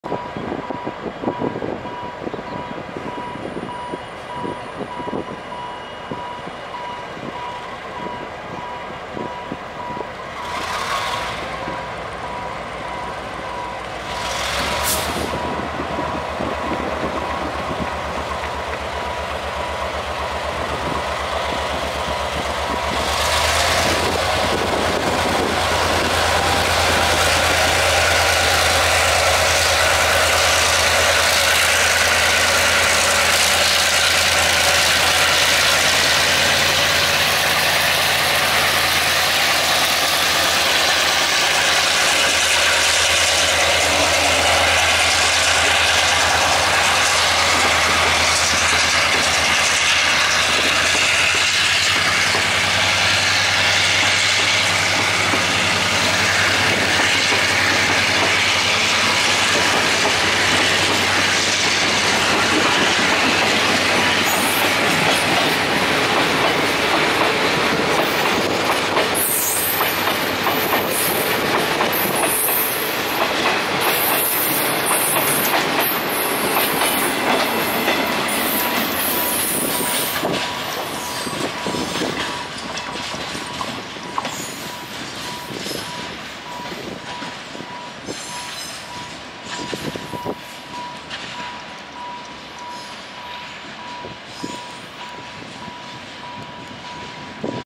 05/05/2022 台鐵 7802次 散裝貨列 本務R153柴電機車(逆牽) 回送R159柴電機車 嘉義出發 / TRA EMD G22-CUs leading manifest train
05/05/2022
台鐵
7802次 散裝貨列
本務R153柴電機車(逆牽) 回送R159柴電機車
嘉義出發
TRA EMD G22-CUs  (long hood forward) leading manifest train,long hood foward,Chaiyi departure